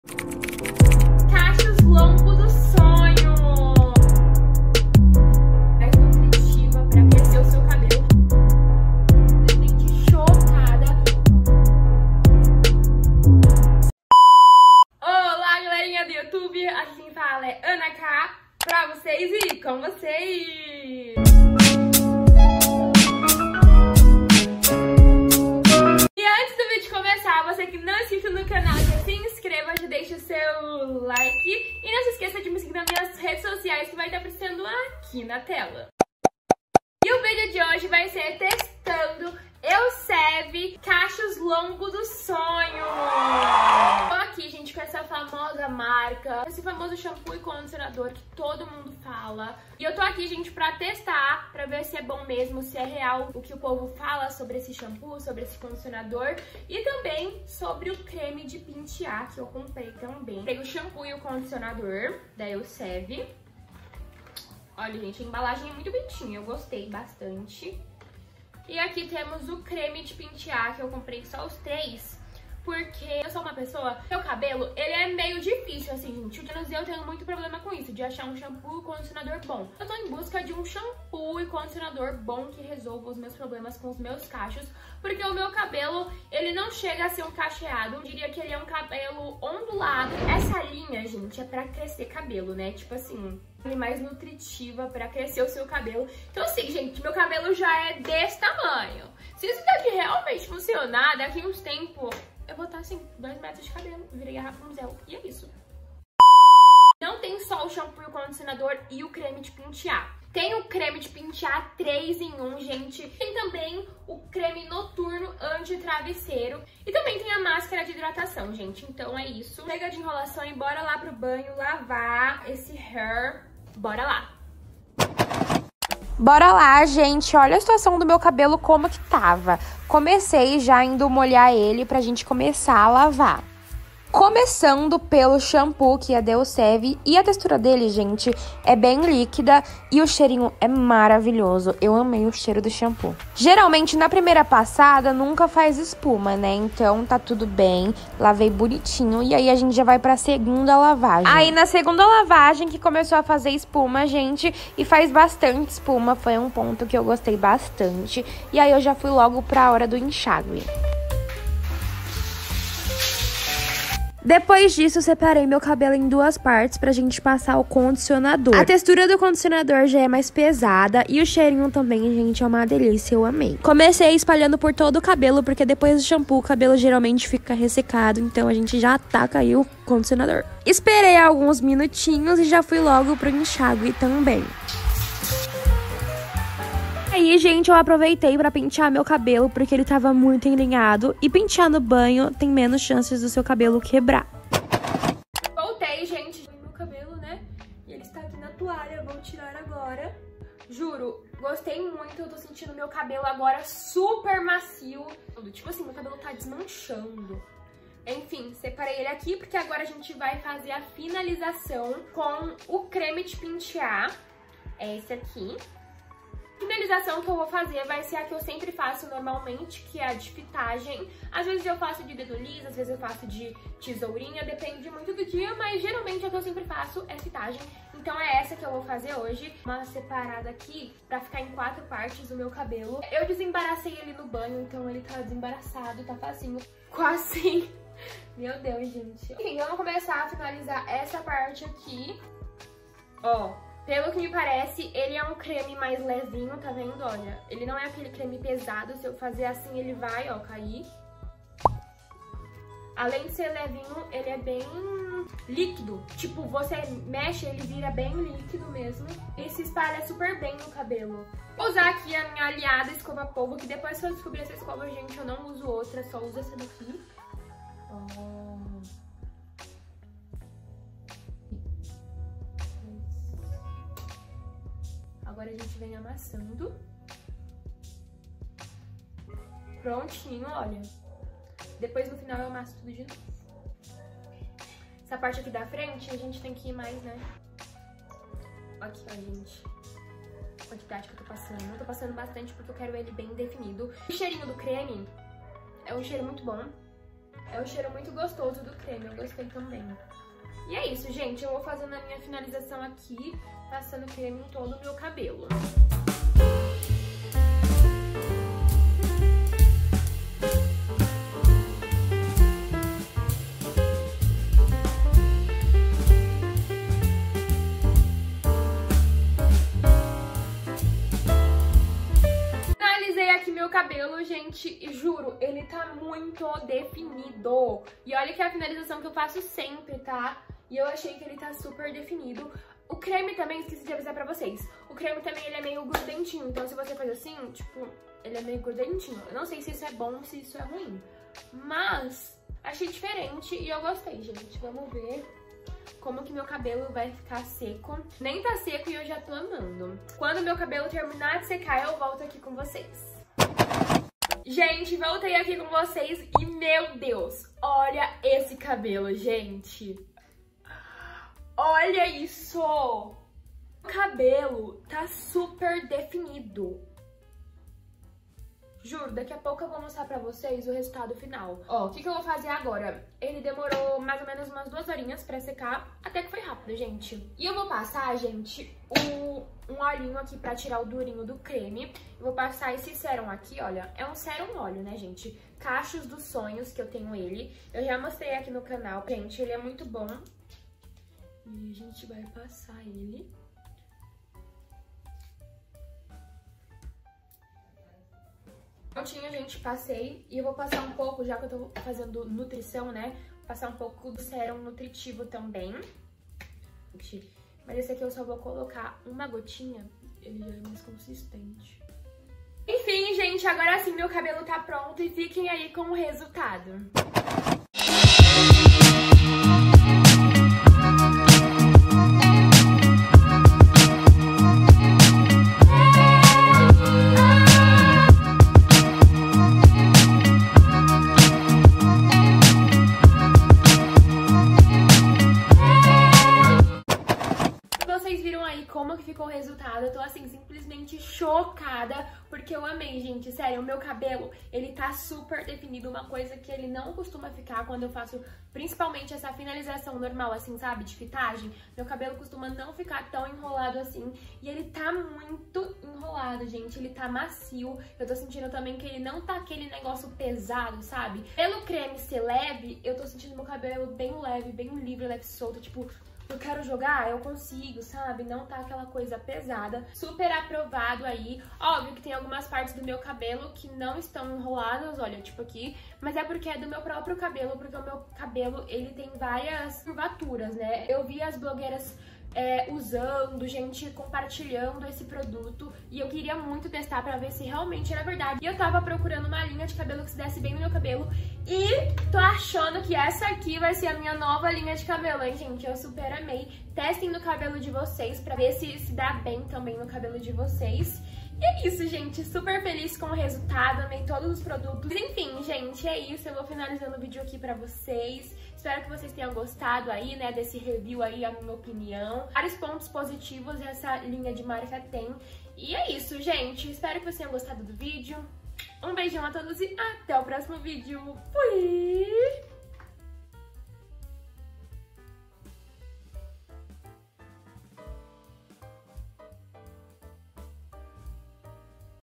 TESTEI ELSEVE CACHOS LONGOS DOS SONHOS | será que vale a pena?
Olá galerinha.
No vídeo de hoje testei a famosa linha da elseve cachos longo dos sonhos e falei o que eu achei. 👍🏻